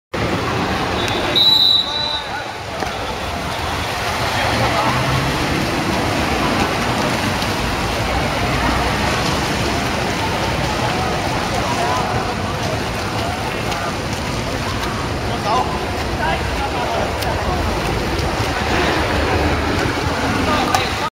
夏天的水球錦標賽 20120722 - 6
Organized by HKASA and subvented by LCSD
香港業餘游泳總會主辦
康樂及文化事務署資助
Summer National & Junior Water Polo Championships 2012
二零一二年夏季青少年水球錦標賽
Date: 22nd July, 2012 (12:06)
日期：二零一二年七月二十二日﹝12時06分﹞
Venue: Wanchai training Pool
地點：灣仔訓練池